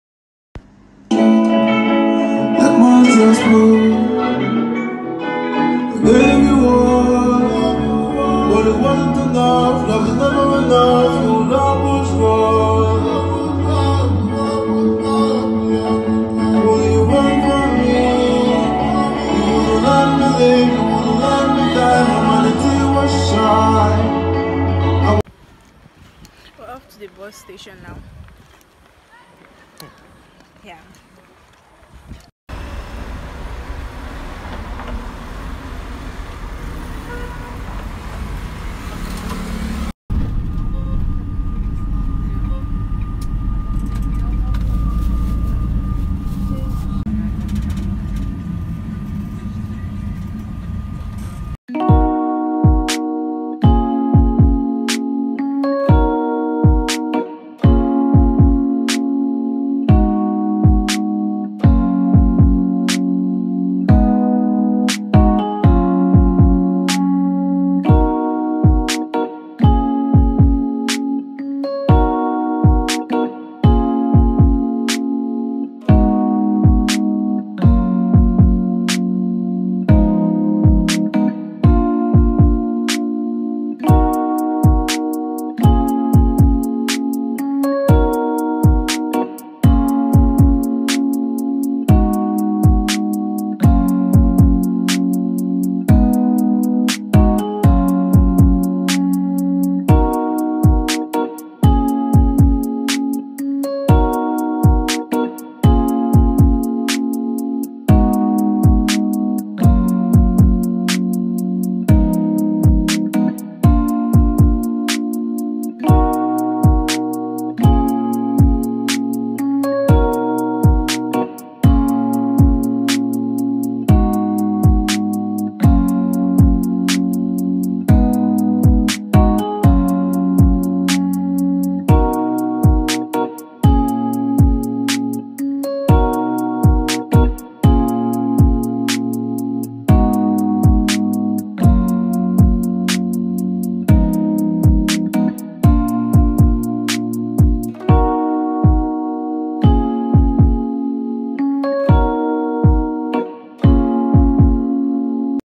Mini vlog| Girne American university during summer| international student| North Cyprus 🇨🇾
Hey loves, this is a previous vlog I didn’t post